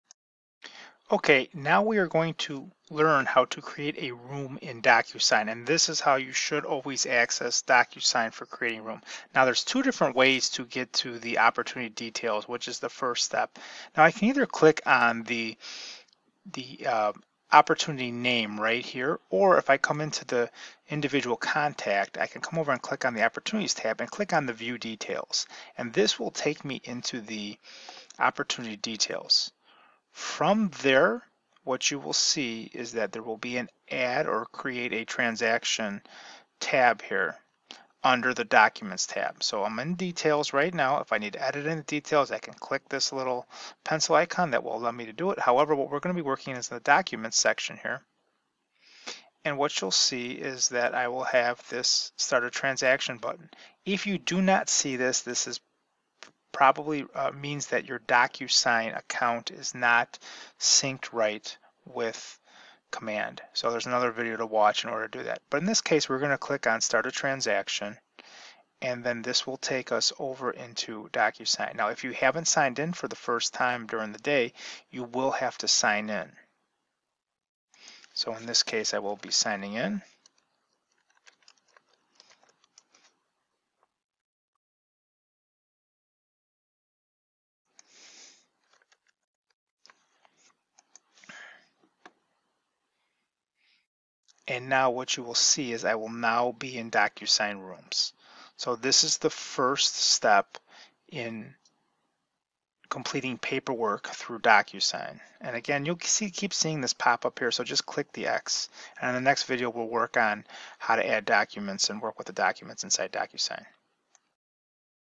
How to Create an Room in Docusign from KW Command
In this video we will work on creating a room in docusion by clicking Start a Transaction in the details tab of the opportunity documents section which is found by either clicking on the opportunity name or clicking on view details from the opportunity of the contact view.